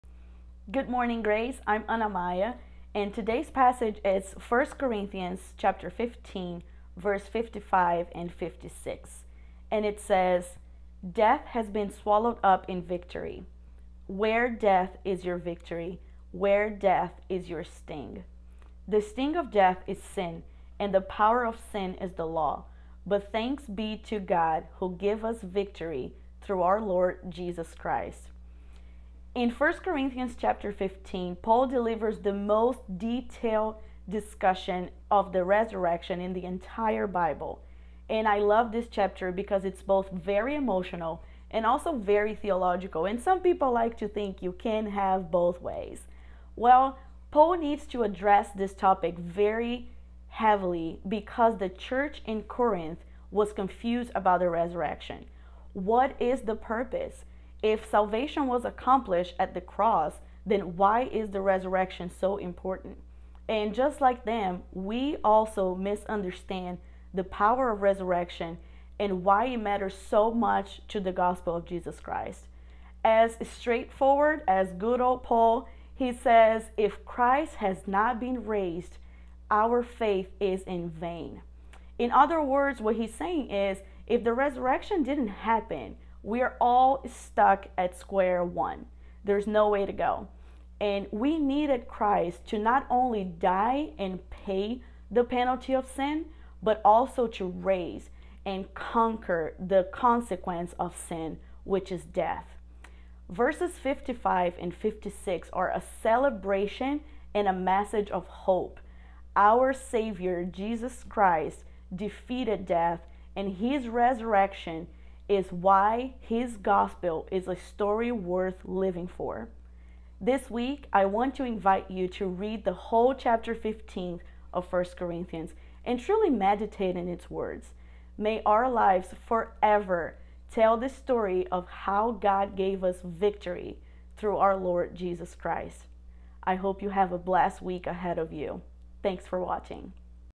Why is Resurrection Important?
A story worth living for. Anna walks us through 1 Corinthians 15, as Paul provides clarity about the Resurrection's importance to the church of Corinth.

If you feel led to support this ministry and help us continue to impact lives worldwide, please